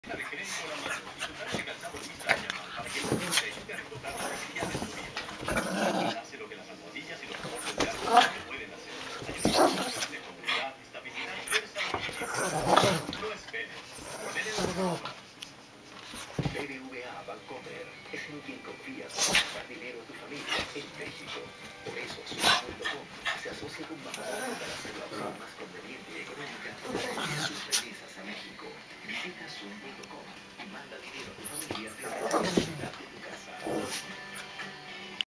Tibetan Spaniel dog fight
On one corner, furious Sassy 'fluffy tail'... on the other corner, Sookie 'sharp teeth'... at stake, the rights to ride in the front seat, let the best bitch win